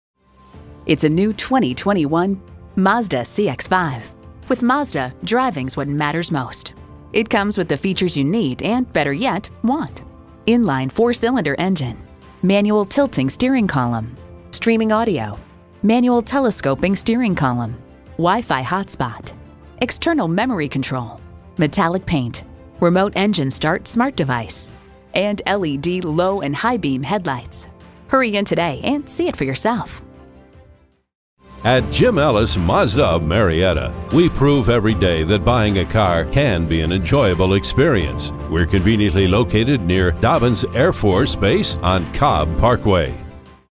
New 2021 Mazda CX-5 Marietta Atlanta, GA Z61986 - SOLD
Year: 2021
Make: Mazda
Model: CX-5
Trim: Sport
Engine: SKYACTIVA® 2.5L 4-Cylinder DOHC 16V
Transmission: 6-Speed Automatic
Color: Red
Interior: Black
Stock #: Z61986
VIN: JM3KFABMXM0322820

Address:
1141 Cobb Parkway Southeast